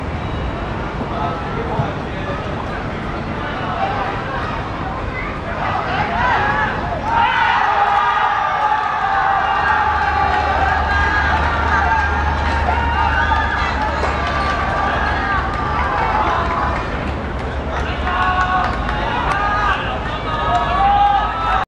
補時29秒出現的頭槌絶殺入球(2018.10.13.元朗學界足球甲組)精選一刻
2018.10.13.屯門鄧肇堅運動塲   宏信(黃衫)